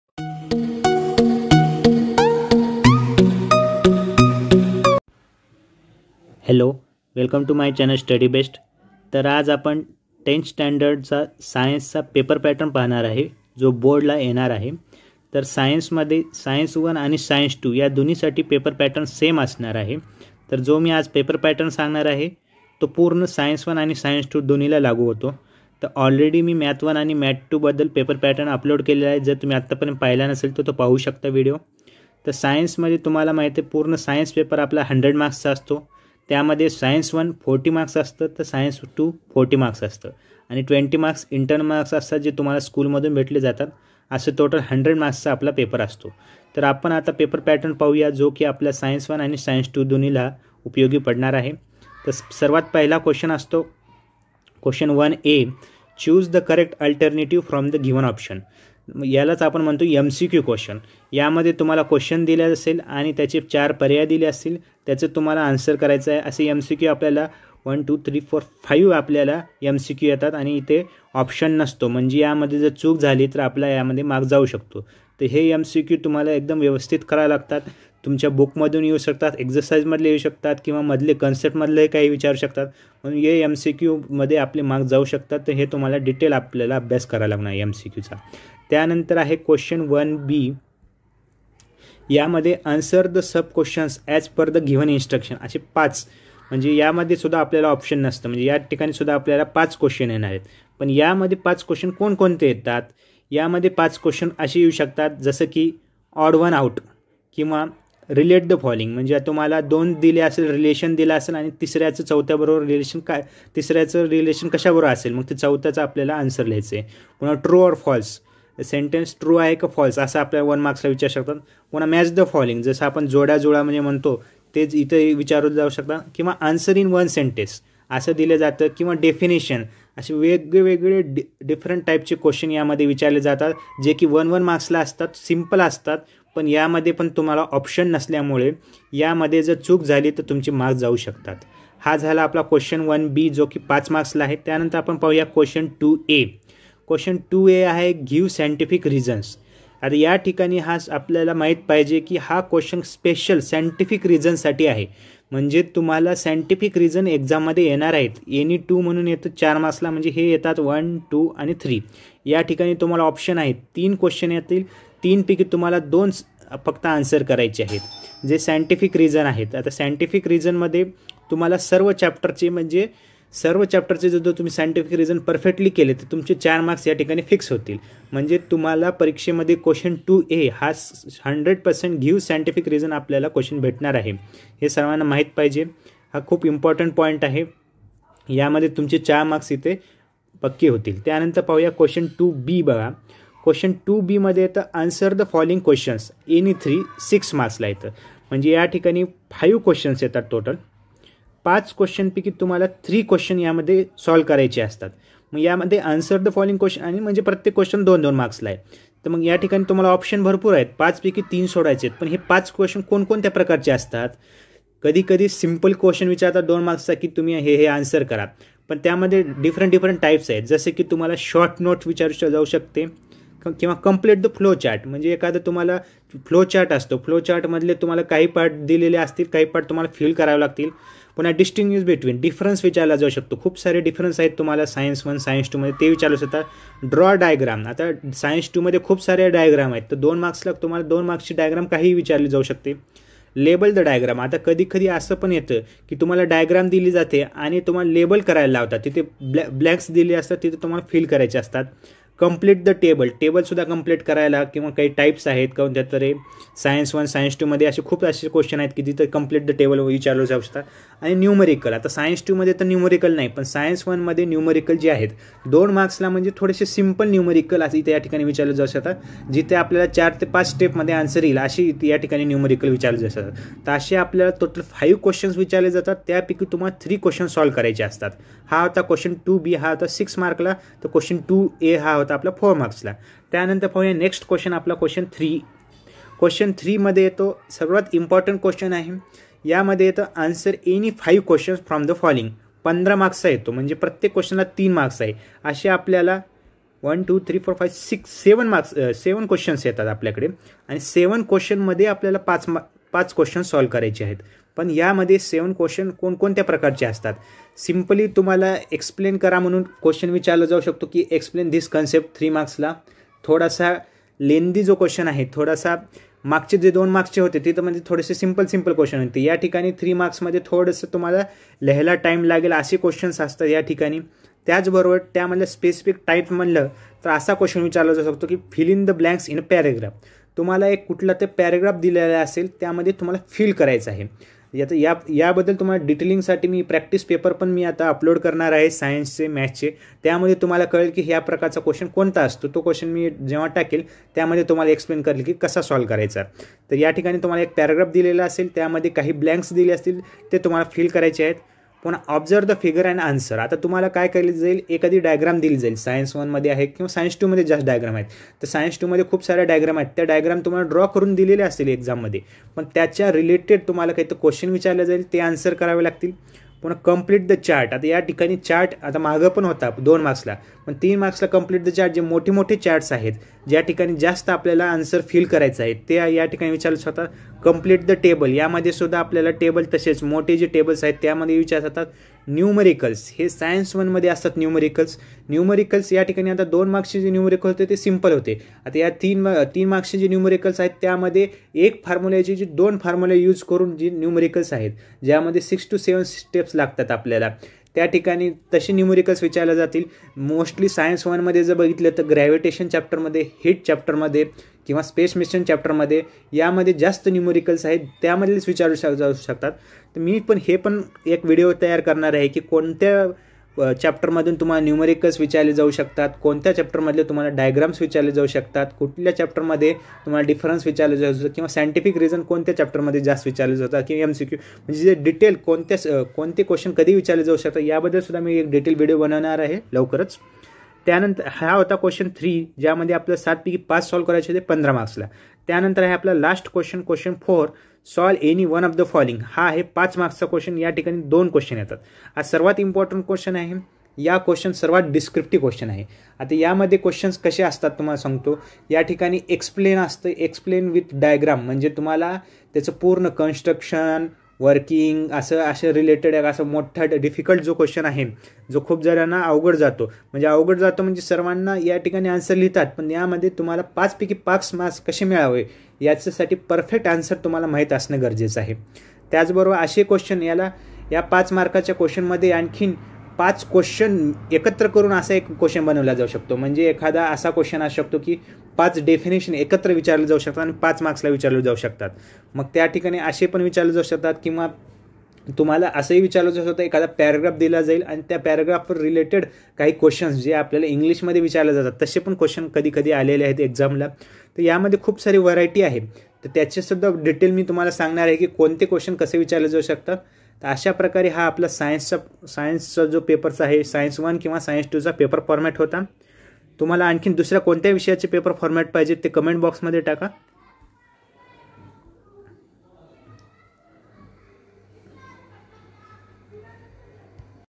Paper Pattern Of Science | Format Of Board's Question Paper | Class10
hi friends,  👋
In this video you will get Paper Pattern Of Science Format Of Board's Question Paper  😀

@Study Best